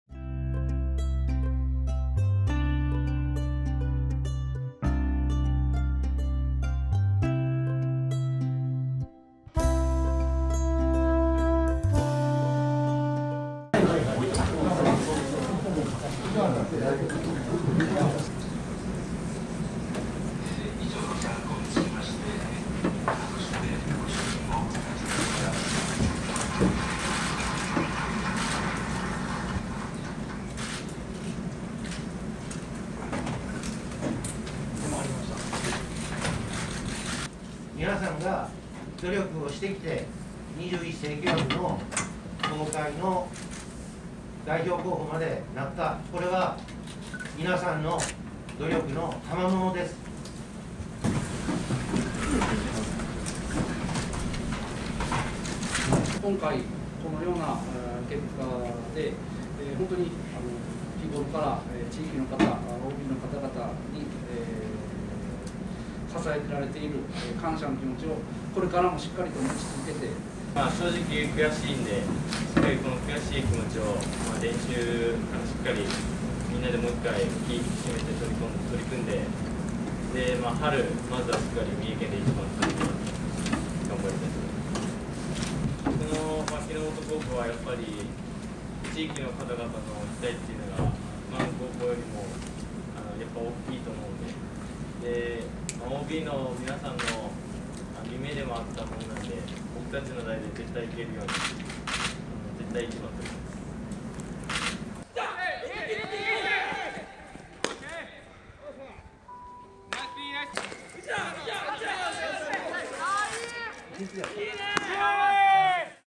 木本高野球部、２１世紀枠吉報は届かず 悔しさバネに夏へ
第９５回選抜高校野球大会の出場校が２７日、発表された。三重県勢では東海地区の２１世紀枠候補校となった木本高（熊野市）に期待が寄せられていたが、県勢初の選出とはならなかった。（森雅貴撮影）